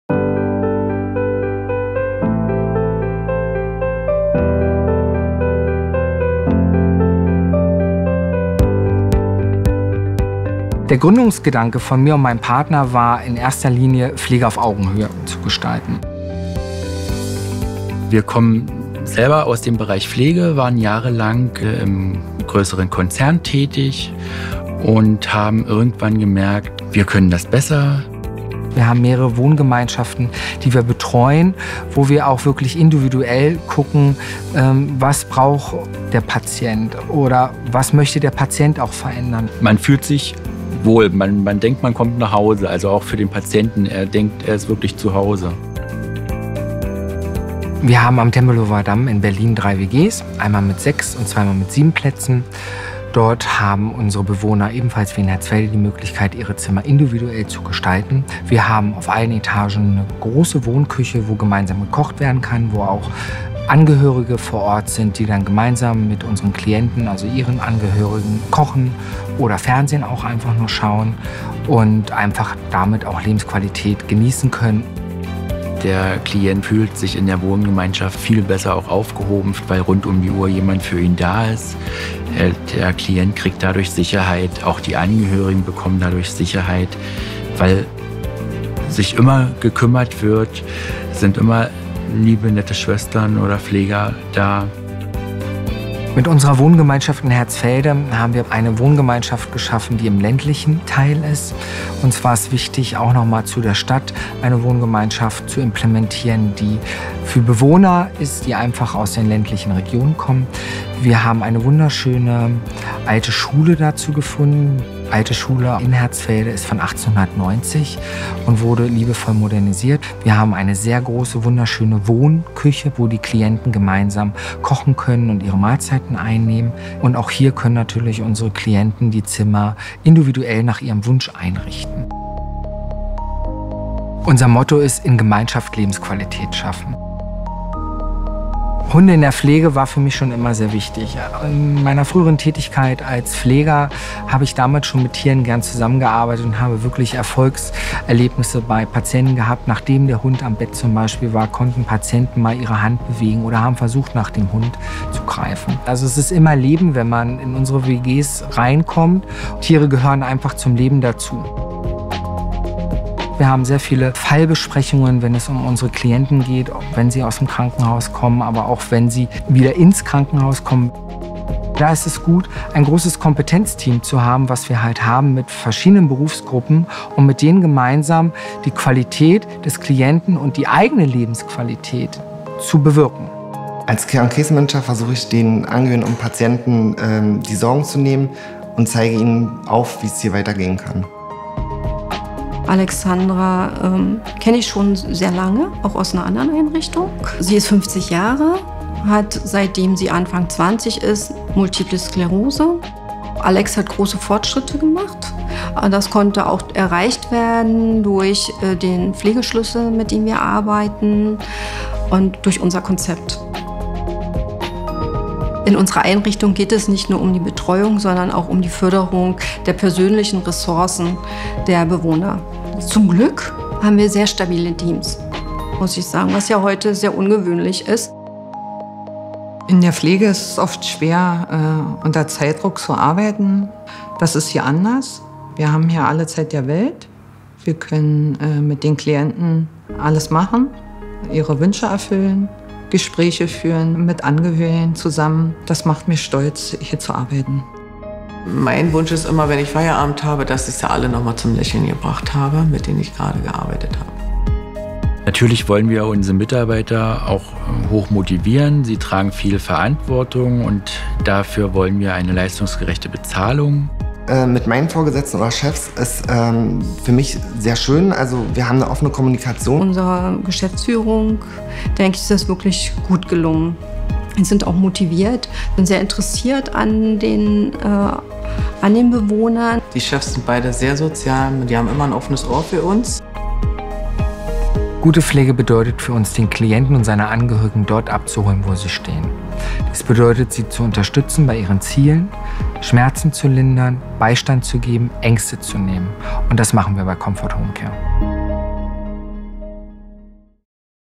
COMFORT Homecare Imagefilm HD
COMFORT Homecare - ein Intensivpflegedienst in Berlin und Brandenburg mit einer besonderen Philosophie. Eine starke Gemeinschaft schafft hier ein Zuhause mit hoher Lebensqualität für intensivpflichtige Klienten.